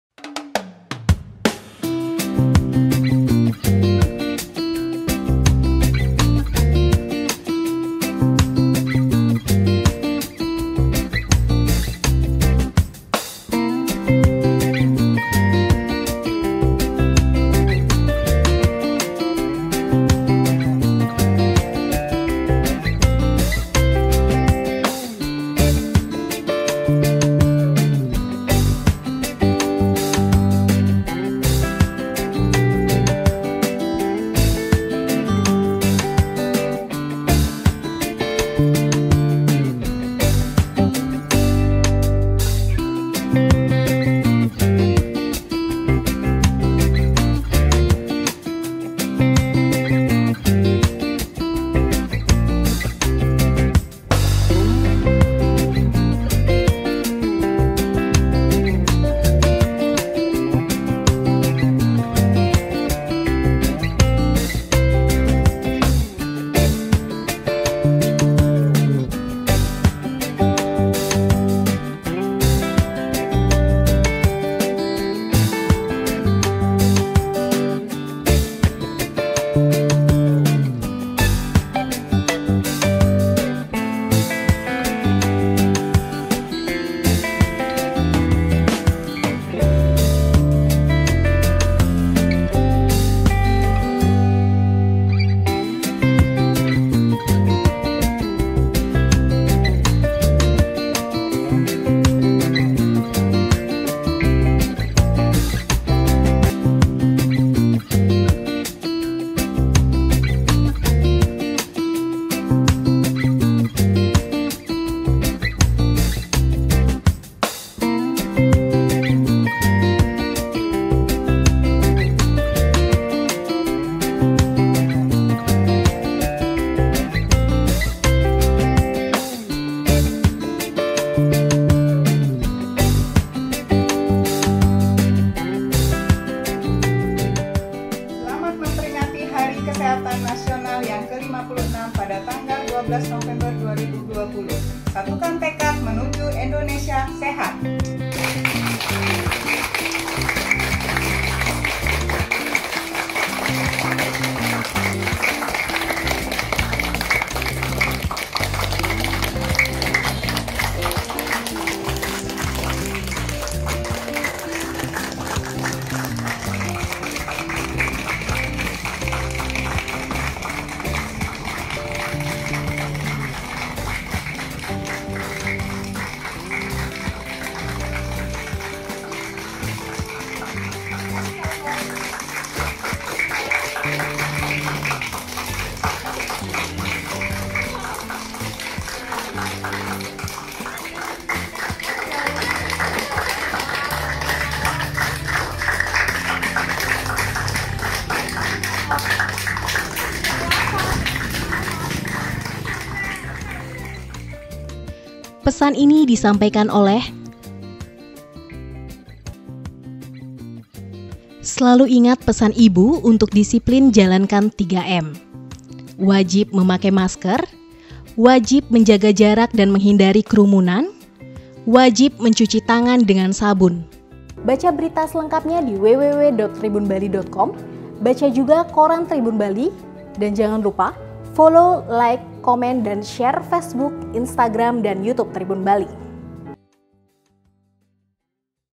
Peringati Hari Kesehatan Nasional ke-56, Puskesmas 3 Densel Bagikan Ribuan Masker
Tema Hari Kesehatan Nasional tahun ini yang jatuh pada Kamis (12/11/2020) adalah Satukan Tekad Menuju Indonesia Sehat.
Peringatan HKN ke-56 tahun ini berbeda dengan tahun sebelumnya.
Dimana tahun ini pandemi Covid-19 melanda sebagian besar dunia termasuk Indonesia.
Dalam rangka memperingati Hari Kesehatan Nasional (HKN) ke-56, Puskesmas 3 Denpasar Selatan (Densel) merayakan dengan membagikan masker kain dan bertepuk tangan serentak.

***Catatan Redaksi: Mari cegah dan perangi persebaran Covid-19. Tribun Bali mengajak seluruh Tribunners untuk selalu menerapkan protokol kesehatan dalam setiap kegiatan.
Ingat Pesan Ibu: Memakai Masker, Mencuci Tangan, dan Menjaga Jarak

(IAG)